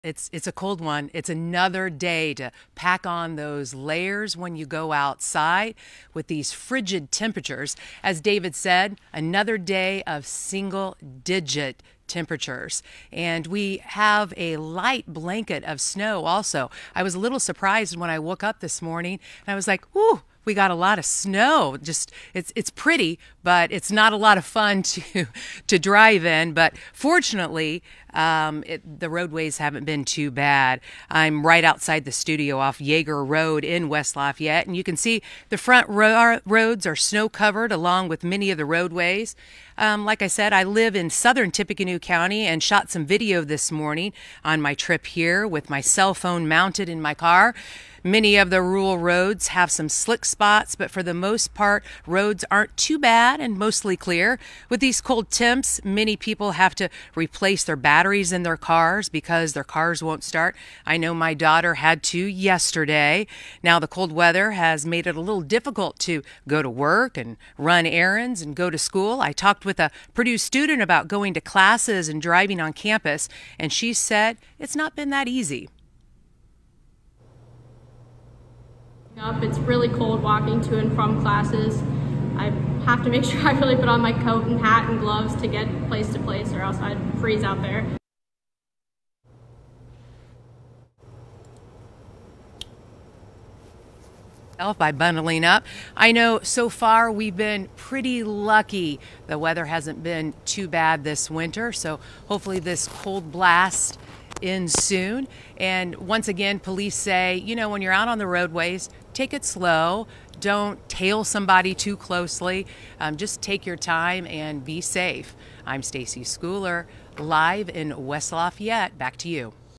Live: Snow and extreme cold temperatures
News 18's Stacy Schooler is live outside the studio this morning in West Lafayette braving the cold weather.